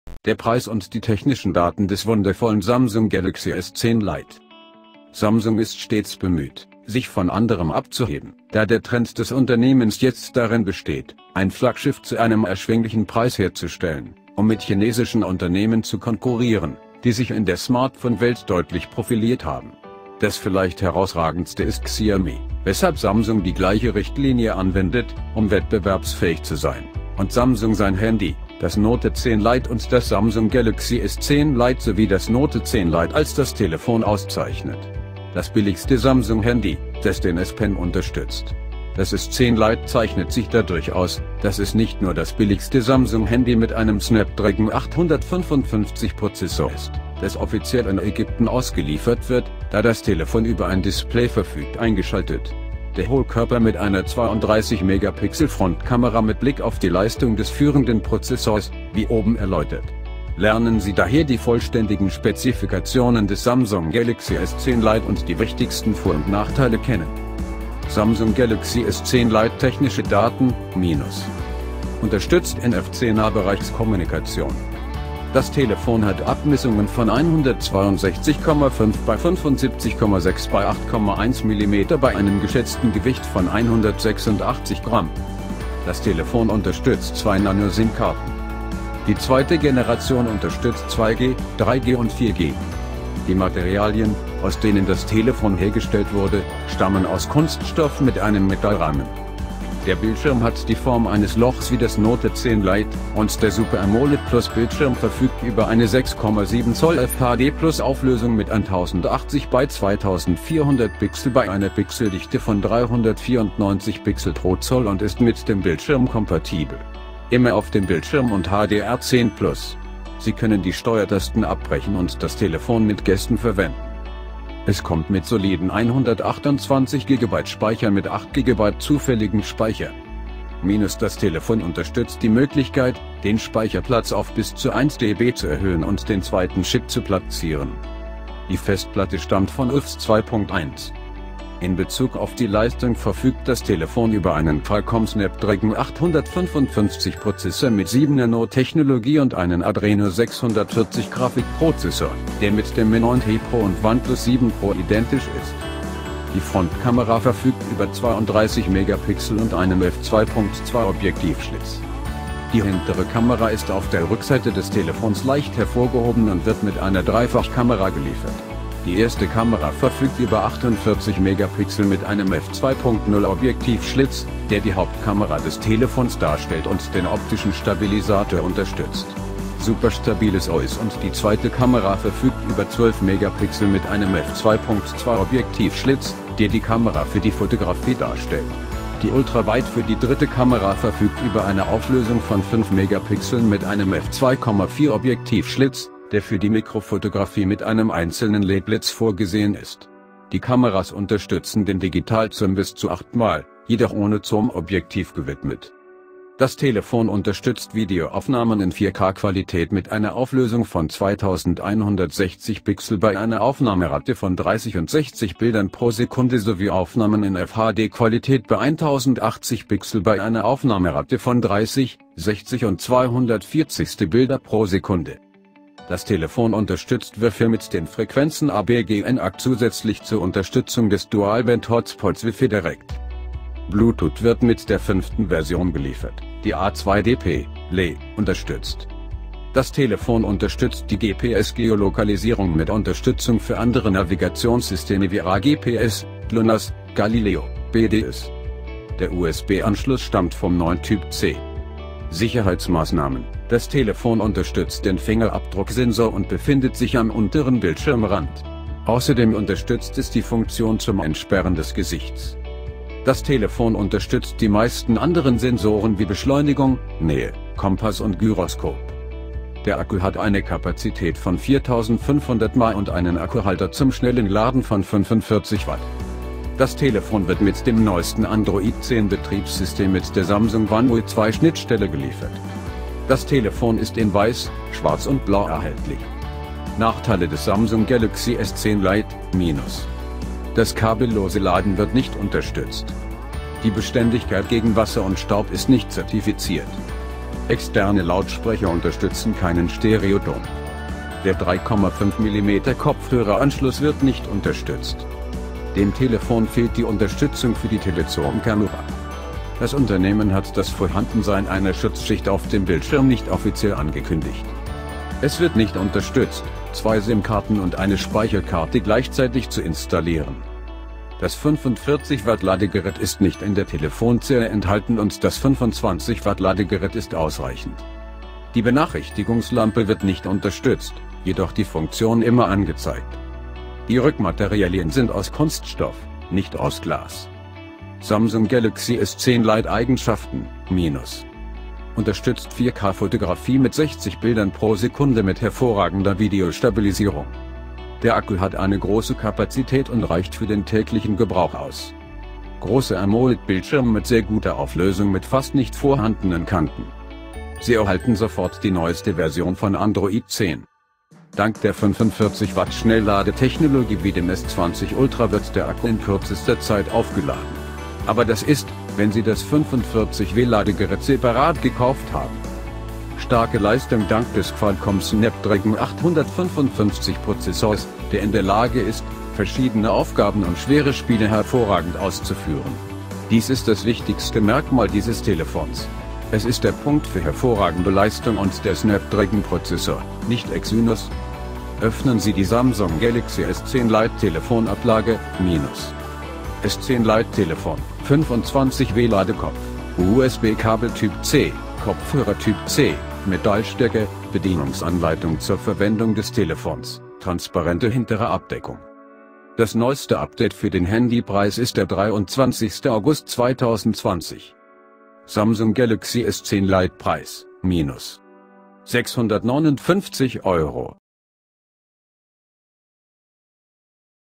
Der Preis und die technischen Daten des wundervollen Samsung Galaxy S10 Lite
Samsung ist stets bemüht, sich von anderen abzuheben, da der Trend des Unternehmens jetzt darin besteht, ein Flaggschiff zu einem erschwinglichen Preis herzustellen, um mit chinesischen Unternehmen zu konkurrieren, die sich in der Smartphone-Welt deutlich profiliert haben. Das vielleicht herausragendste ist Xiaomi, weshalb Samsung die gleiche Richtlinie anwendet, um wettbewerbsfähig zu sein, und Samsung sein Handy, das Note 10 Lite und das Samsung Galaxy S10 Lite sowie das Note 10 Lite als das Telefon auszeichnet. Das billigste Samsung-Handy, das den S Pen unterstützt. Das S10 Lite zeichnet sich dadurch aus, dass es nicht nur das billigste Samsung-Handy mit einem Snapdragon 855-Prozessor ist, das offiziell in Ägypten ausgeliefert wird, da das Telefon über ein Display verfügt eingeschaltet. Der Hohlkörper mit einer 32-Megapixel-Frontkamera mit Blick auf die Leistung des führenden Prozessors, wie oben erläutert. Lernen Sie daher die vollständigen Spezifikationen des Samsung Galaxy S10 Lite und die wichtigsten Vor- und Nachteile kennen.

Samsung Galaxy S10 Lite Technische Daten: -
Unterstützt NFC-Nahbereichskommunikation.
Das Telefon hat Abmessungen von 162,5 x 75,6 x 8,1 mm bei einem geschätzten Gewicht von 186 Gramm.
Das Telefon unterstützt zwei Nano Sim-Karten.
Die zweite Generation unterstützt 2G, 3G und 4G.
Die Materialien, aus denen das Telefon hergestellt wurde, stammen aus Kunststoff mit einem Metallrahmen.
Der Bildschirm hat die Form eines Lochs wie das Note 10 Lite, und der Super AMOLED Plus-Bildschirm verfügt über eine 6,7-Zoll-FHD + -Auflösung mit 1080 x 2400 Pixel bei einer Pixeldichte von 394 Pixel pro Zoll und ist mit dem Bildschirm kompatibel. Immer auf dem Bildschirm und HDR10 +. Sie können die Steuertasten abbrechen und das Telefon mit Gesten verwenden.
Es kommt mit soliden 128 GB Speicher mit 8 GB zufälligen Speicher.
- Das Telefon unterstützt die Möglichkeit, den Speicherplatz auf bis zu 1 TB zu erhöhen und den zweiten Chip zu platzieren.
Die Festplatte stammt von UFS 2.1.
In Bezug auf die Leistung verfügt das Telefon über einen Qualcomm Snapdragon 855-Prozessor mit 7-Nano-Technologie und einen Adreno 640-Grafikprozessor, der mit dem Mi 9T Pro und OnePlus 7 Pro identisch ist.